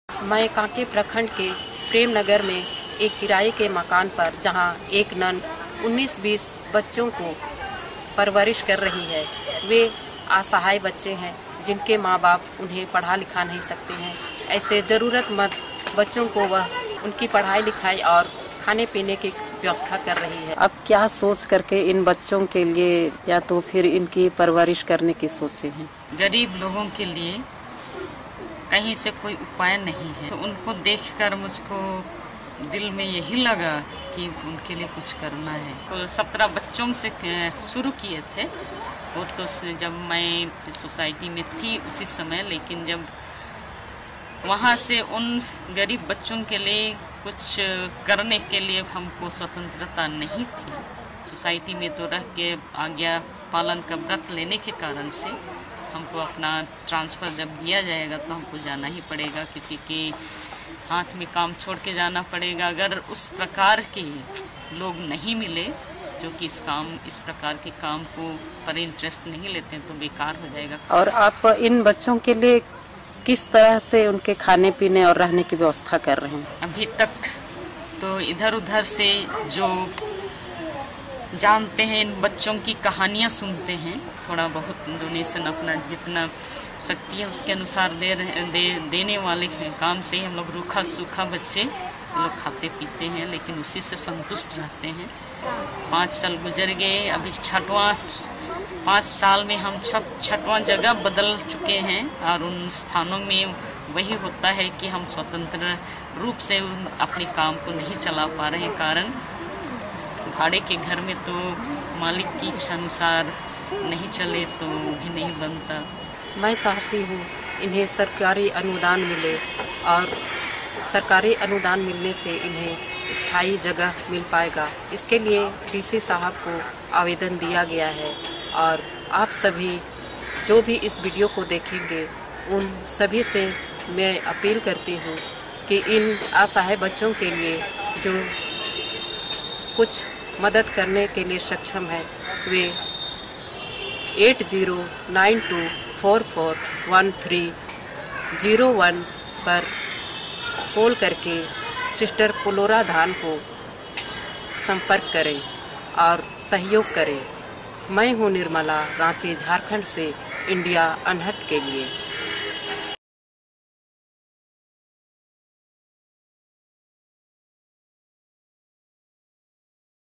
One Billion Rising: Eager to Read
A small rented house in Ranchi, Jharkhand fills up with the sound of children laughing and playing. For the 30 odd residents of this house things could have been different. They could have been out on the streets without food or education. But in Sister Pulora Dan's care, they have both.

Seeing that there were children whose parents could not afford to provide for them, she started taking care of a handful of children a few years ago. Disillusioned with the lack of support she got from her church, she decided to break away and carry out on her own. There are now about 30 children who live with sister Pulora.

The children are taught Hindi, English and Maths. They are in a safe and nurturing environment which makes up for the distance with their biological family.

"When I went to visit them, the children were so eager to read out from their books to show me that they could read!" says Community Correspondent Nirmala Ekka.

Sister Pulora's work has not been without struggle; she is dependant on donations. For now she has been able to make do as some people provide food and others give money for books. But the lack of funds has meant that there is a certain amount of uncertainty. Nirmala has talked to the State Assembly but the funds they have promised will take time to come through.

Nirmala asks you to contribute to Sister Pulora's cause. You can call her on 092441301.